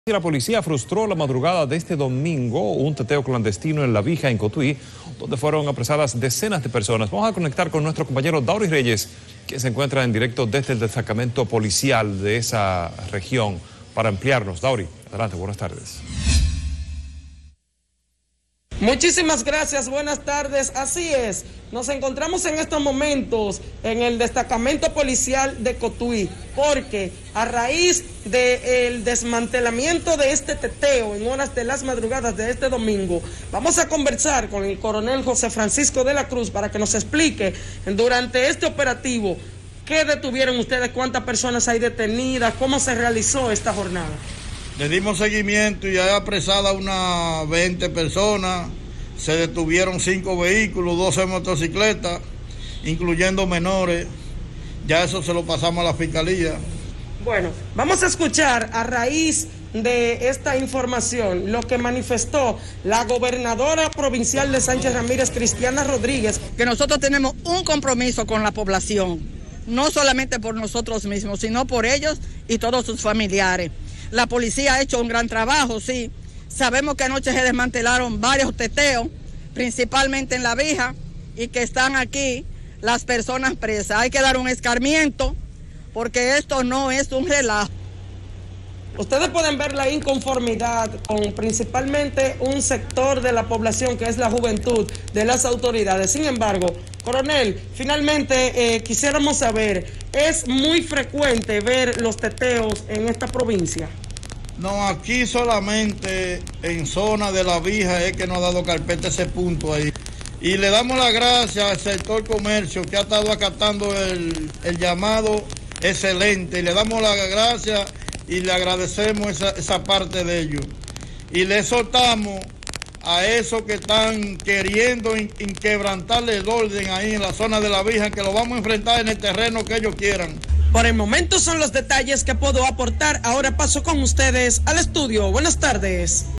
Policía desmantela fiesta clandestina en Cotuí
La Policía Nacional frustró la madrugada de este domingo una fiesta clandestina en La Bija, Cotuí, donde fueron apresadas decenas de personas.